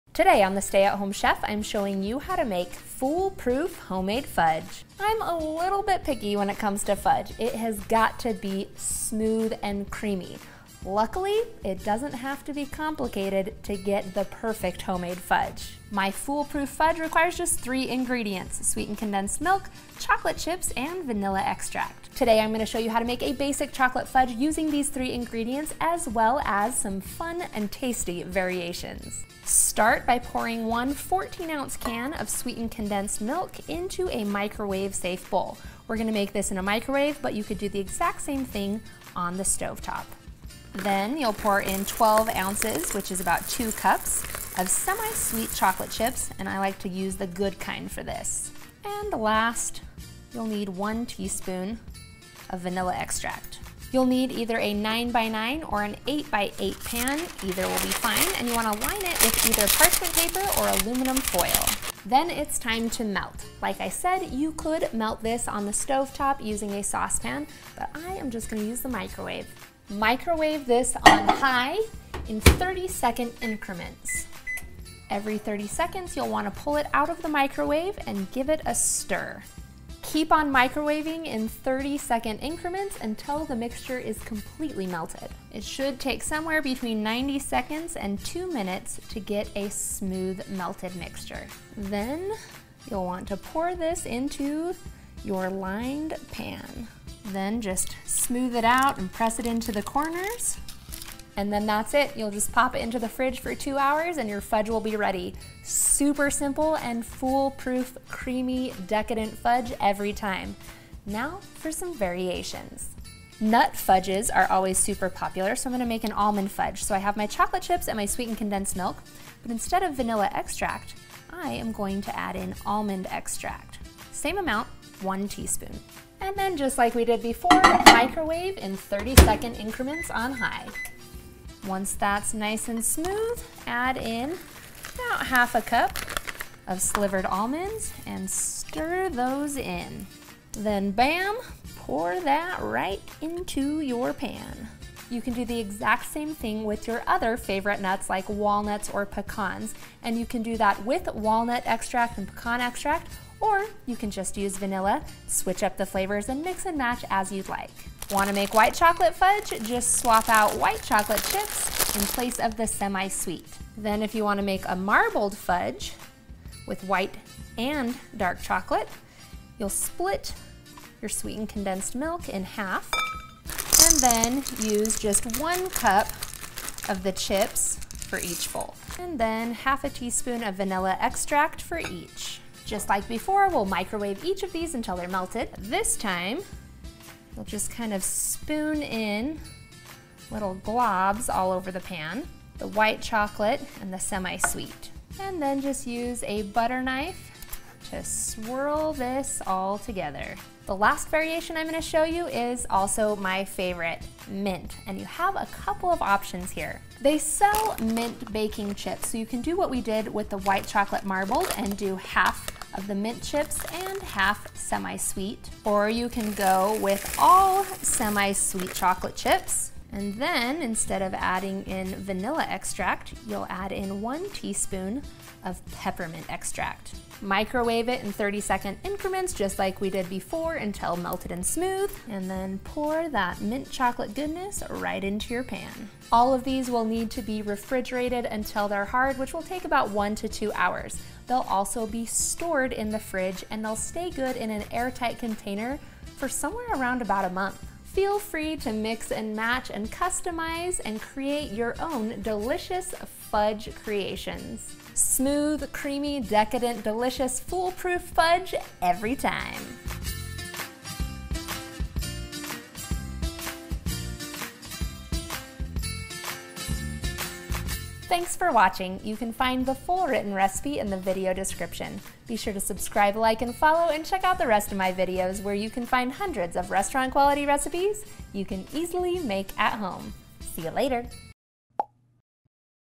Fool-Proof Homemade Fudge
Fool-Proof Homemade Fudge takes just 5 minutes to make and only requires a handful of ingredients for perfectly smooth and creamy fudge every time.

✅Ingredients

• 1 (14 ounce can) sweetened condensed milk
• 2 cups semi-sweet chocolate chips
• 1 teaspoon vanilla extract

✅Instructions

1️⃣ Line an 8x8 pan with parchment paper.

2️⃣ In a large microwave-safe bowl, combine sweetened condensed milk, chocolate chips, and vanilla extract.

3️⃣ Microwave in 30 second increments until melted, stirring in between.

4️⃣ Pour into the prepared pan and spread out with a rubber spatula.

5️⃣ Refrigerate until set, about 2 hours. Remove from pan and cut into small pieces for serving. Store in the fridge.

🥜 Nut Fudges🥜: stir in 1/2 cup roughly chopped nuts (pecan, walnut, or almond). Consider replacing all or part of the vanilla extract with the corresponding nut extract.

🎅🏻 White Chocolate Fudge🎅🏻: replace semi-sweet chocolate chips with white chocolate chips.

🌀 Marbled Fudge🌀: make half of the recipe with white chocolate chips and the other half with semi-sweet chocolate chips. Spoon each into prepared pan to dot the bottom then use a butter knife or toothpick to swirl together.

🍬 Mint Chocolate Fudge🍬: replace vanilla extract with 1 teaspoon peppermint extract.

FACEBOOK: The Stay At Home Chef
PINTEREST: The Stay At Home Chef